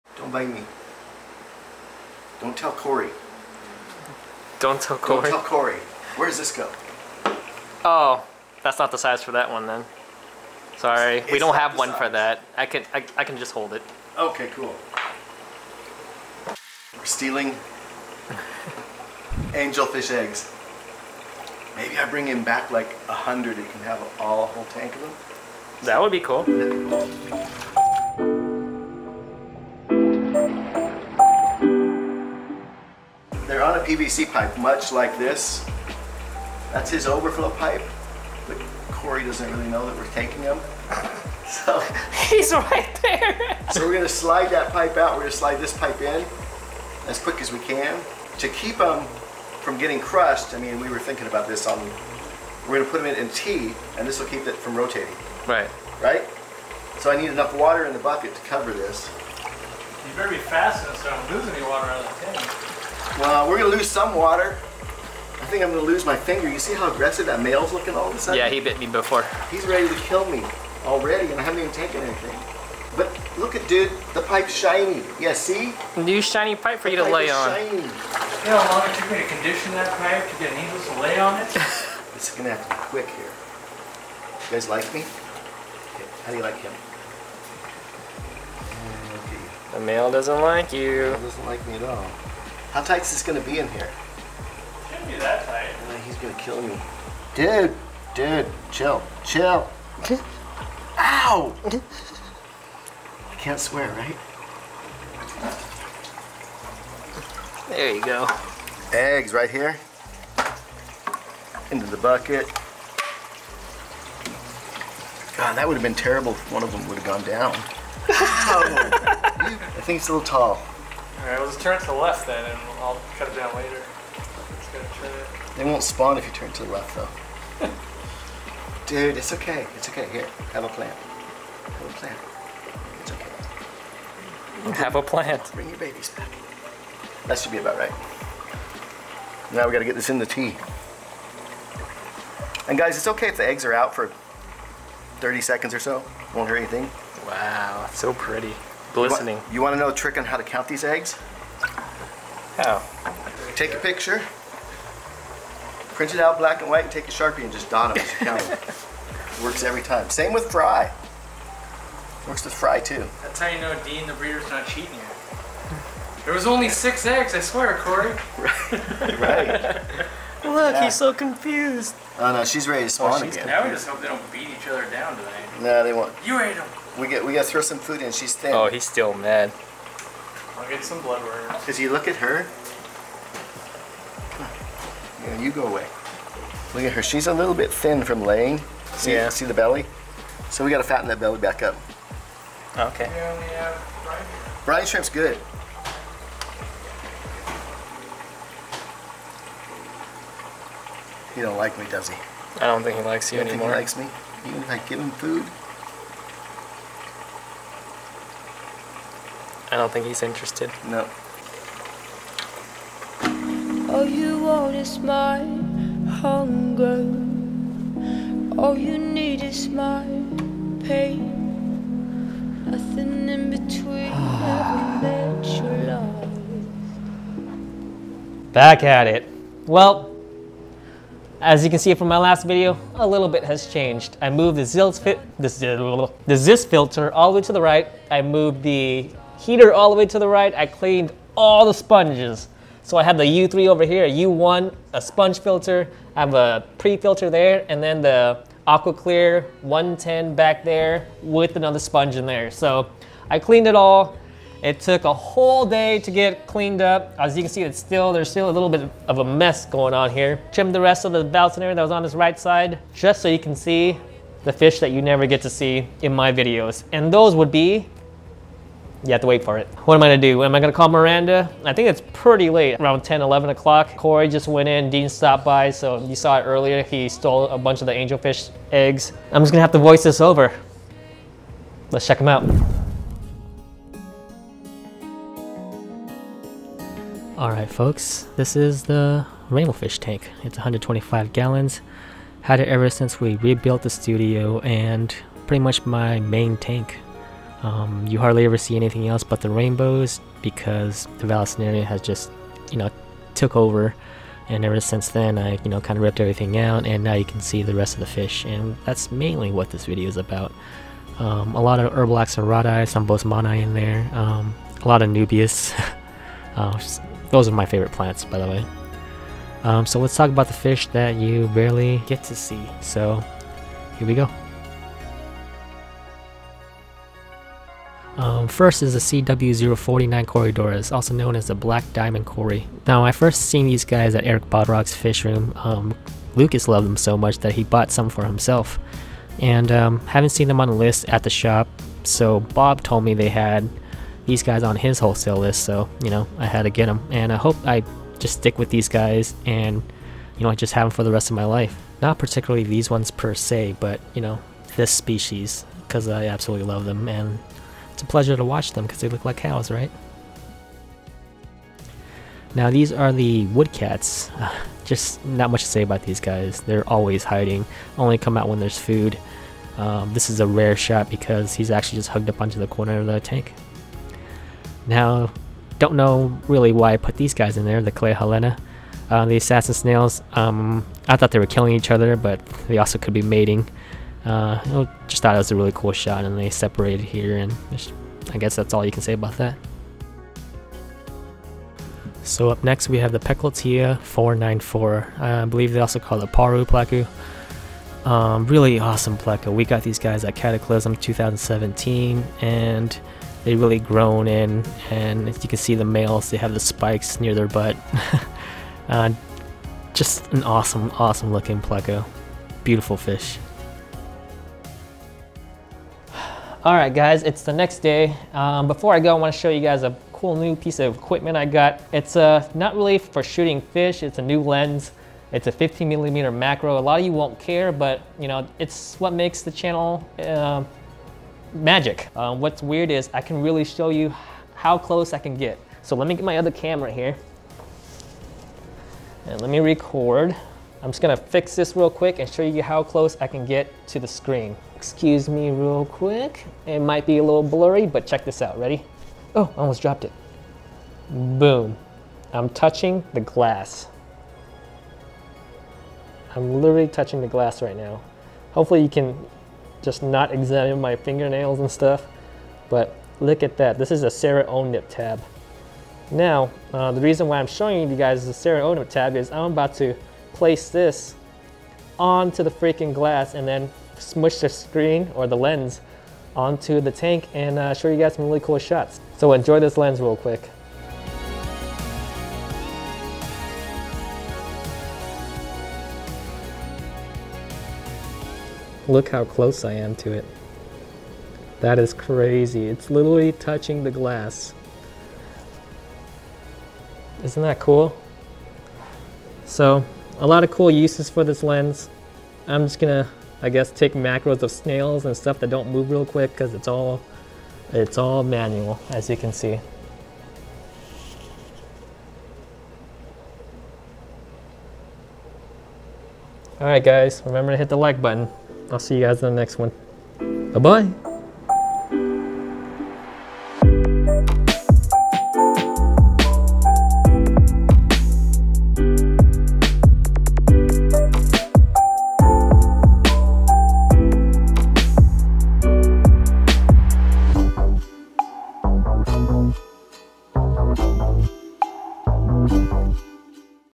The Smallest Fish in My Biggest Aquarium
In this episode, Dean stops by to take some Angelfish eggs home. I clean up my 125 gallon Rainbowfish tank and show you the hidden occupants. I also received a new lens and I show you the capabilities.

My Main Camera
Favorite Fish Lens
Vlogging Lens
Favorite Lens